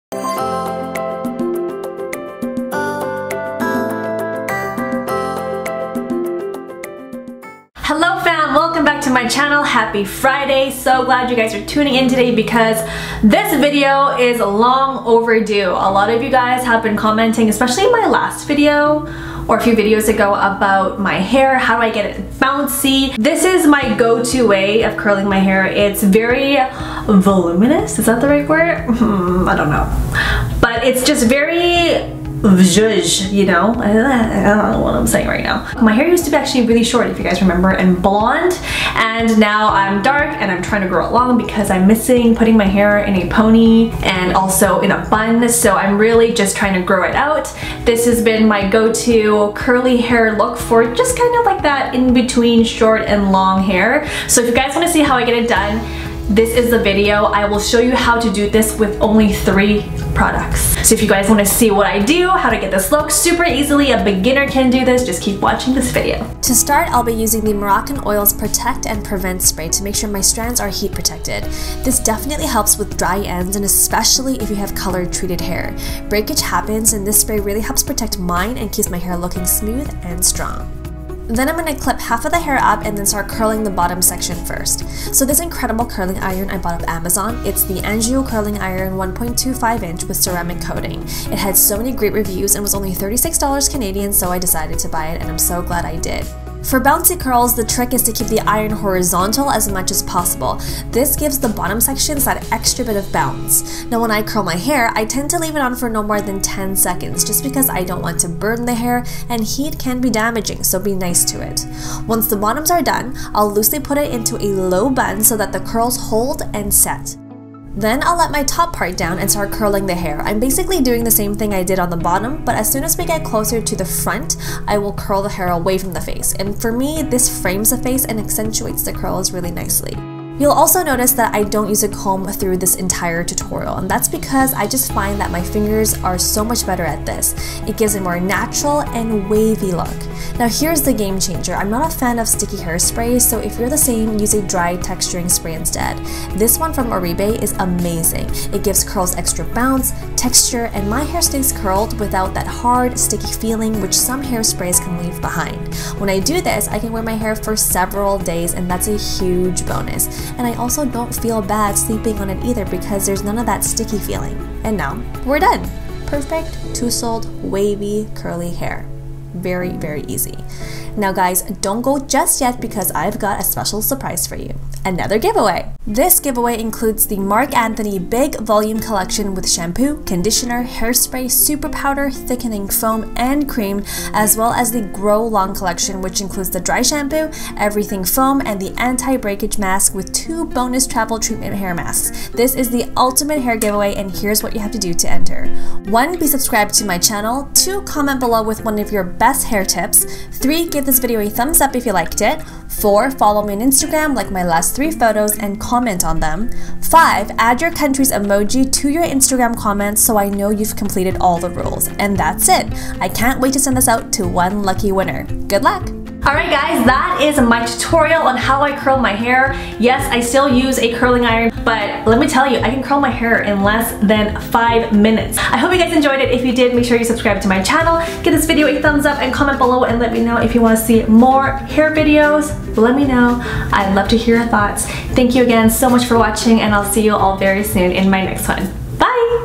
HOW TO: Curl Your Hair For Beginners | chiutips
Got FLAT hair? I show you HOW TO: Curl Your Hair For Beginners. Go from flat and straight hair to beautiful, bouncy, curly hair using just 3 PRODUCTS! I go step by step and show you how to get beautiful curls that last for days!

5. Include your country’s flag (emoji) to all your Instagram comments so I know you completed all the steps!

Good luck!

Contest ends: May 11, 2019

Anjou 1.25 Ceramic Hair Curling Iron